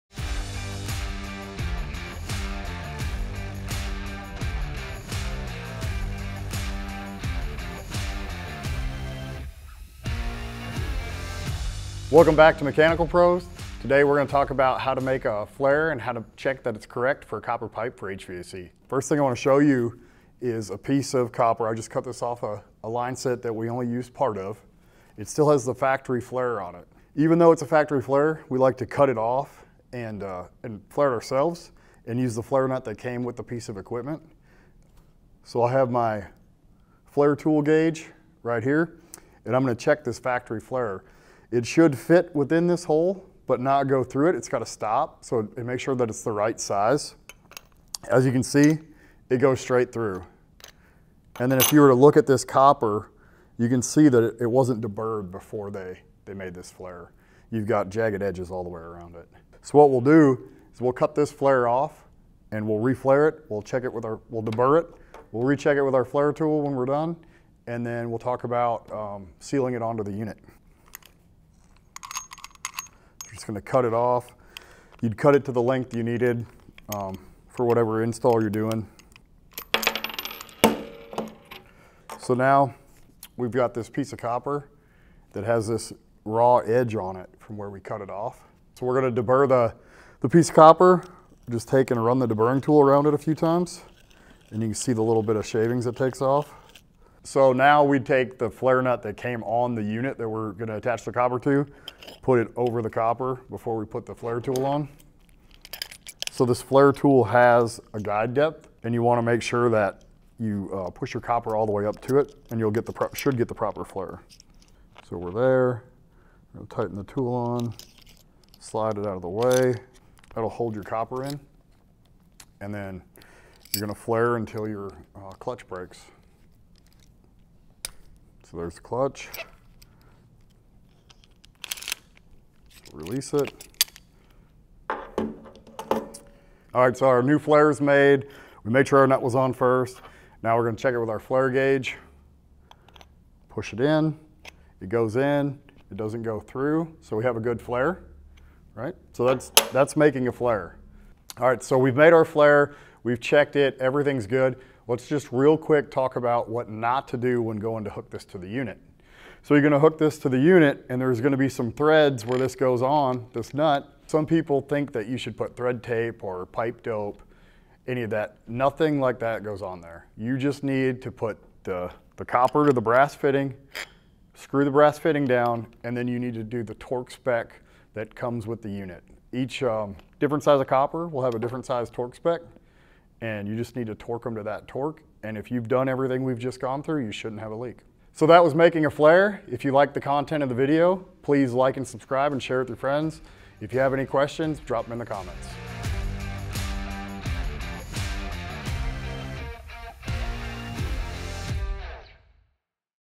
Mastering Copper Pipe Flares: DIY Techniques for HVAC Pros
In our latest video tutorial, we're diving into the art of crafting perfect copper pipe flares for your HVAC projects. Step up your skills as we guide you through the steps of creating flawless flares using a clutch flare tool.

Because when it comes to ensuring a leak-free system, trusting the manufacturer's flares isn't enough. Learn from the pros and avoid common blunders like forgetting to deburr or not checking your finished flare diameter, ensuring your HVAC work stands the test of time.

It's not just about flaring; it's about flaring right. Discover the trade secrets and essential tips in this comprehensive copper pipe flaring demonstration. Elevate your HVAC game today! 🔧👨‍🔧

to learn more about our services.